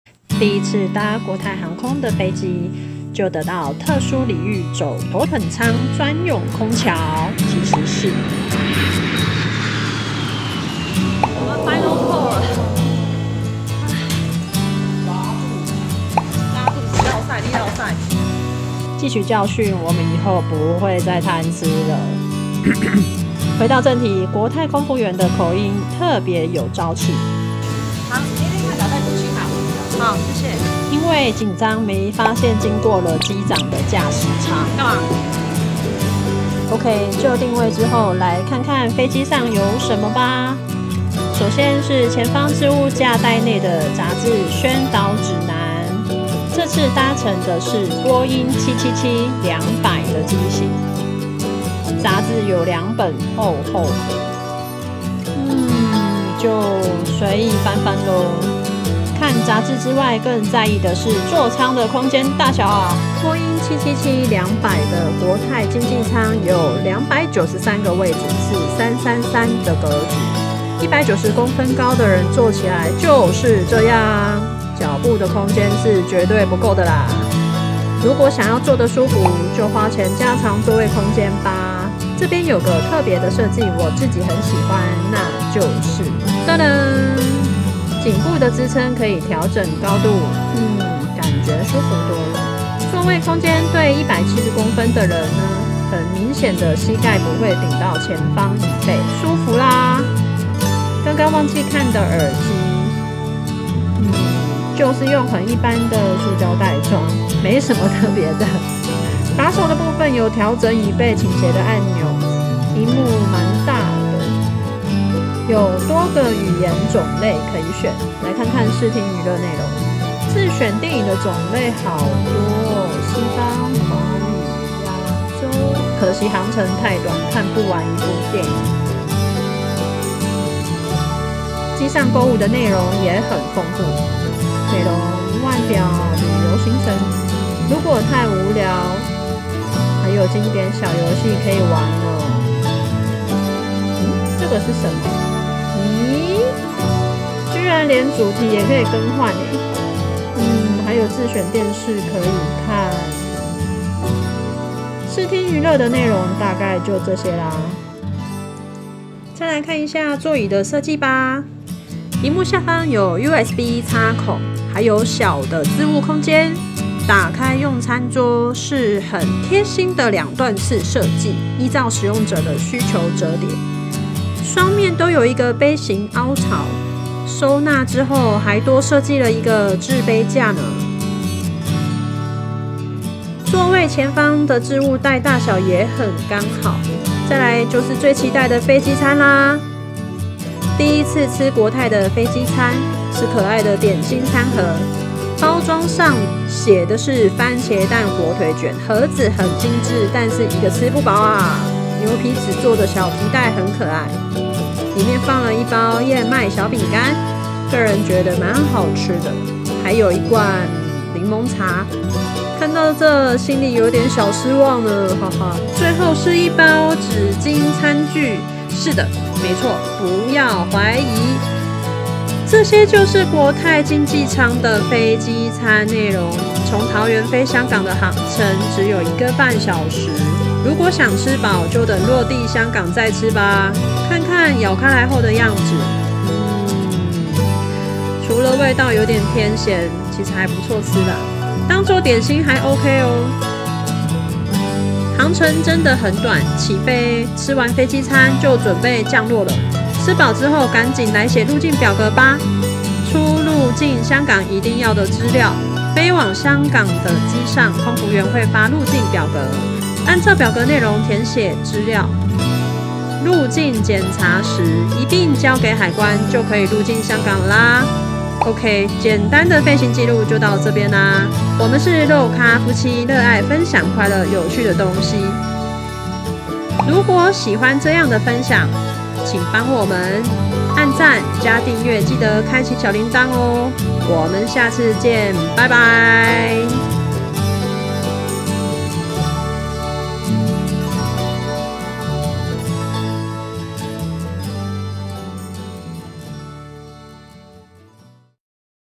國泰航空經濟艙搭乘體驗《台灣-香港》
去了3次香港
第一次搭 國泰航空 波音777 系列
記得在機上向空服員索取香港入境表，港簽的部分要在台灣先辦好(可以線上免費申辦)，或是持 台胞證 就可以快速入境~

最納悶的是...
去、回的飛機餐乍看之下以為是蘋果派
結果都是含有豬肉的捲餅!!

恩? 不是宣導要防疫豬瘟